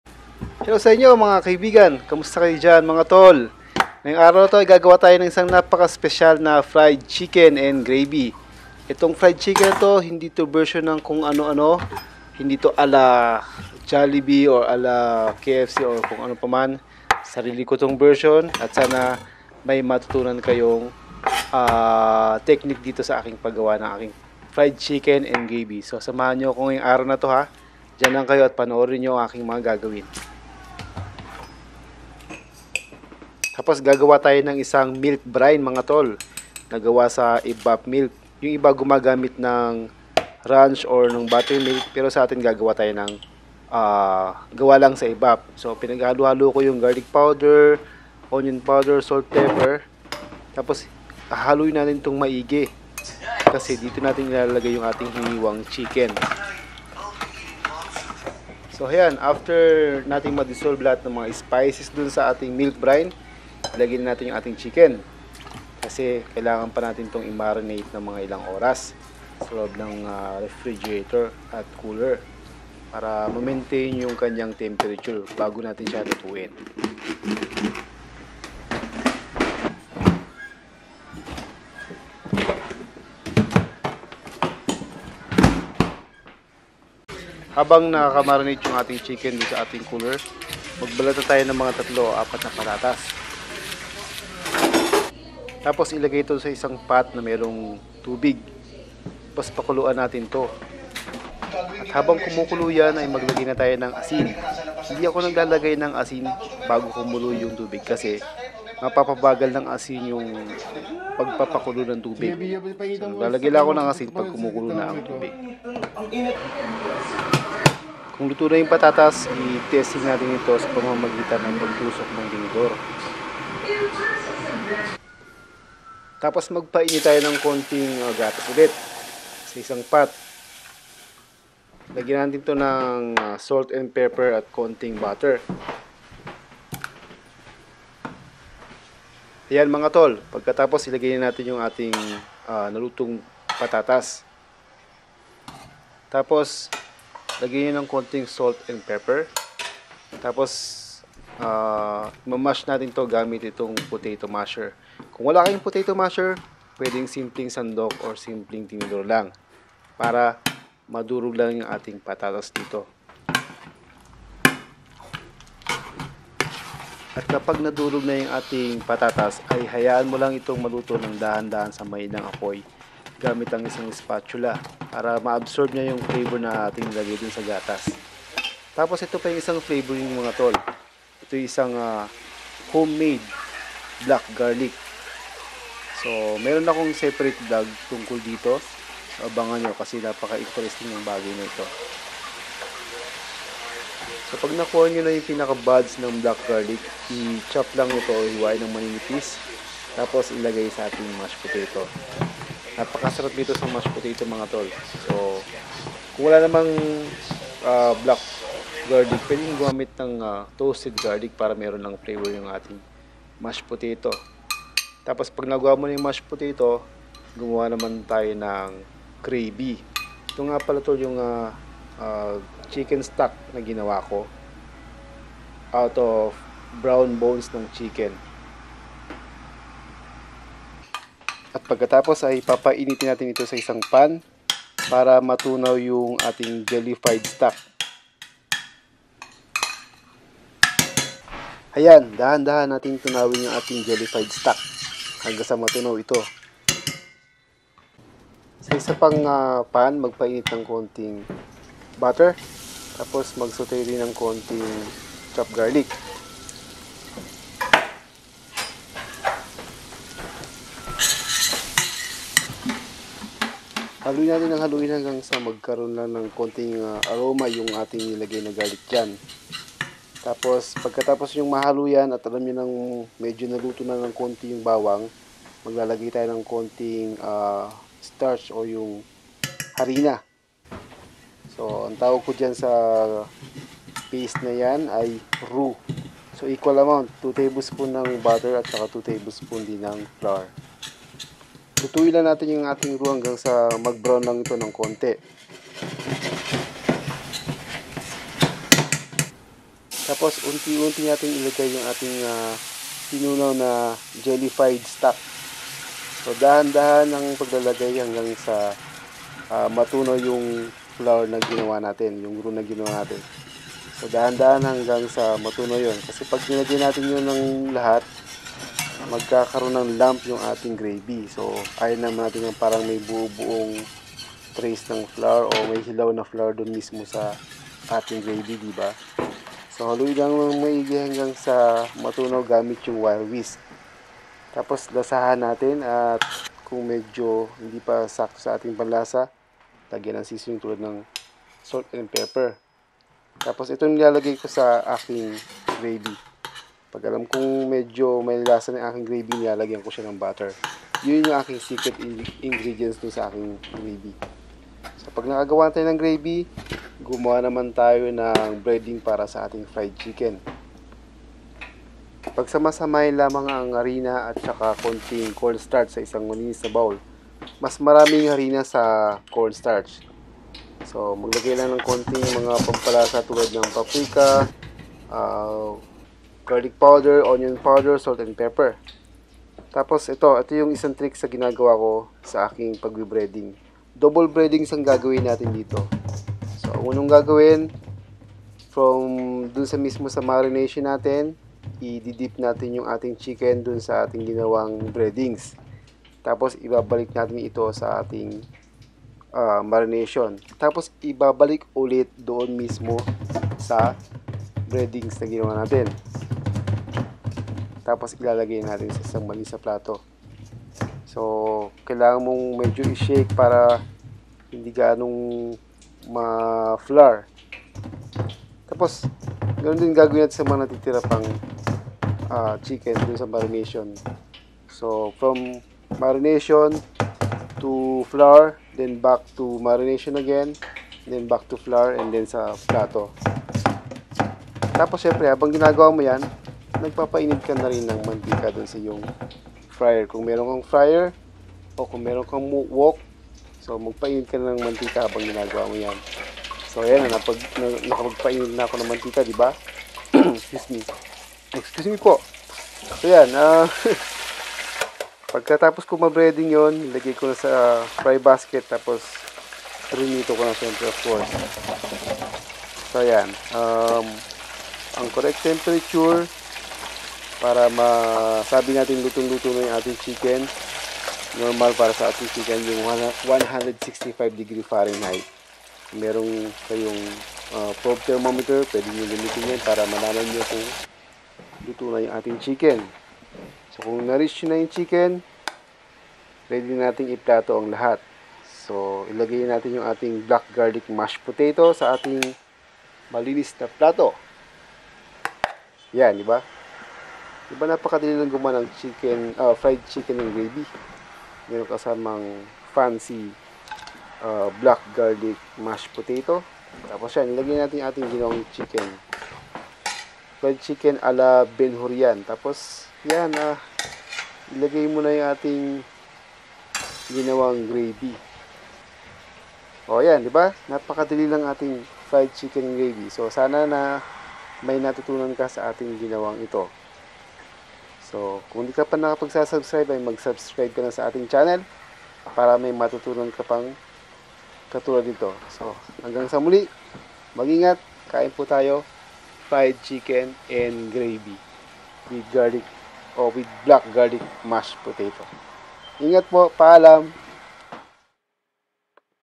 PRITONG MANOK AT BLACK GARLIC MASHED POTATO-MADALI AT SIMPLE LANG
SIMPLE FRIED CHICKEN AND GRAVY WITH BLACK GARLIC MASHED POATATO THAT IS EASY TO FOLLOW.